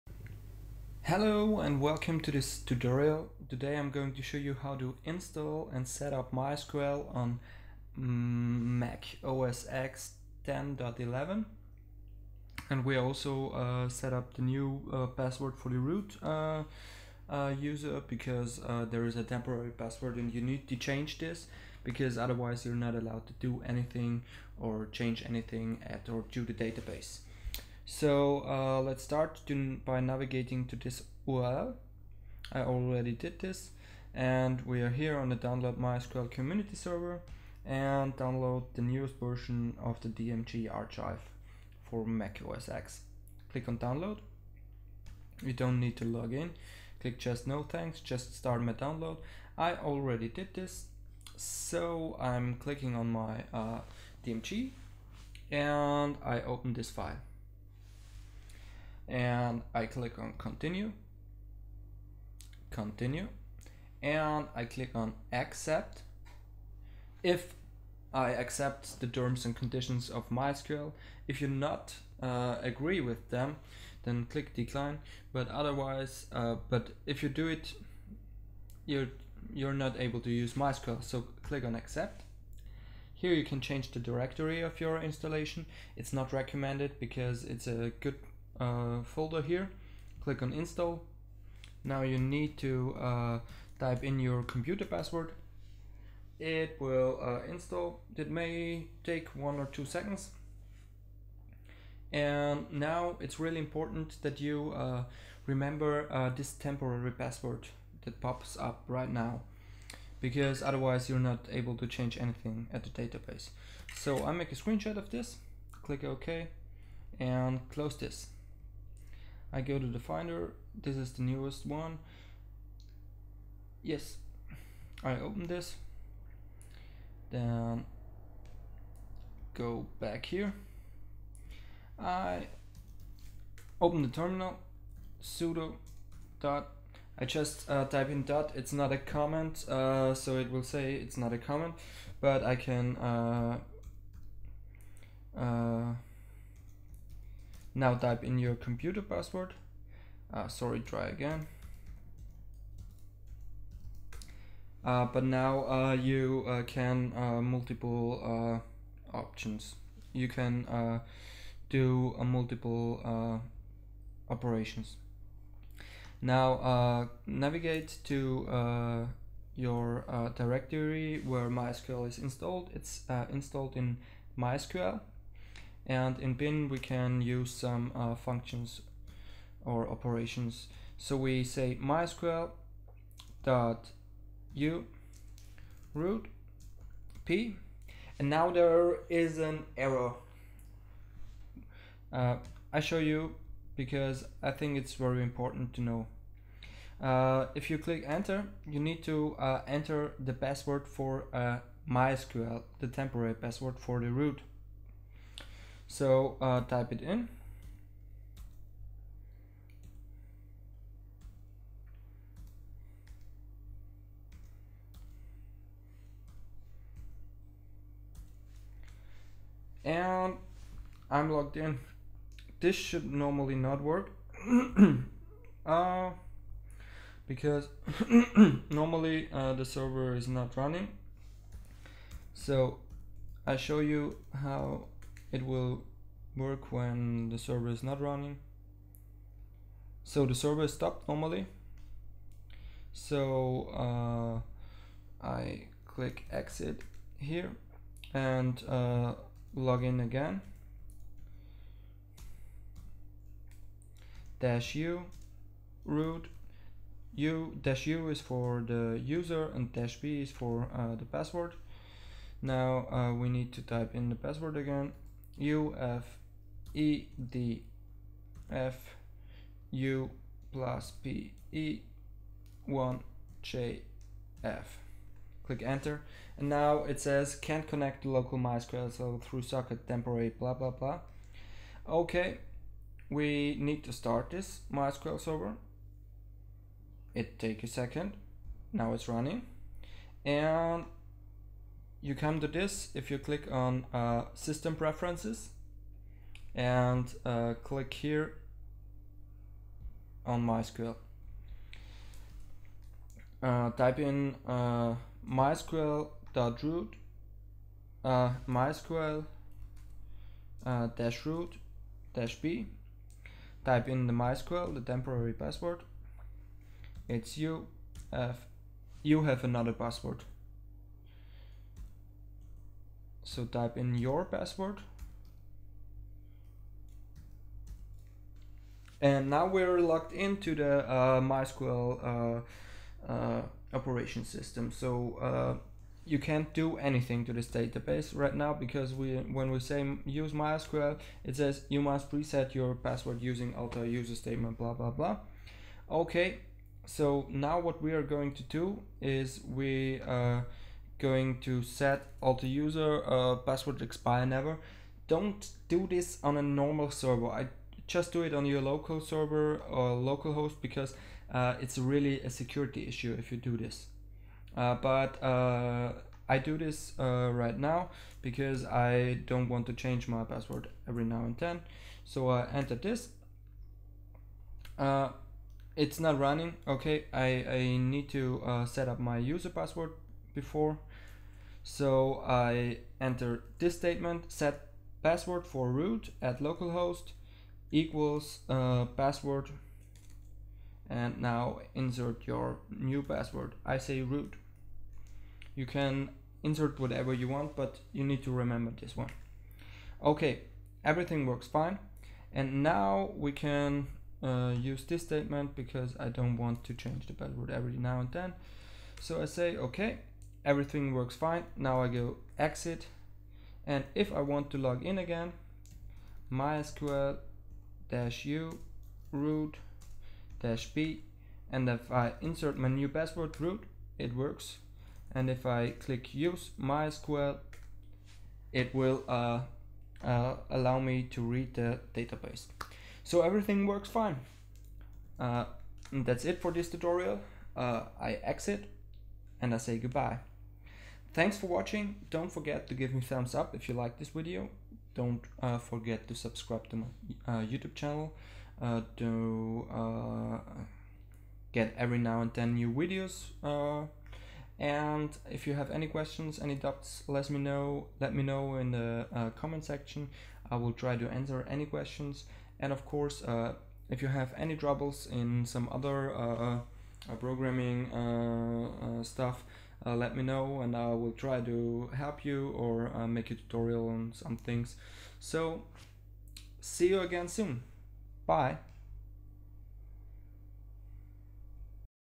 Set up MySQL on Mac OS X 10.11 - Tutorial [ENG]
In this tutorial we will learn how to install and use a MySQL server on Mac OS X el capitan(10.11). Also we reset the temporary user password for the "root" user.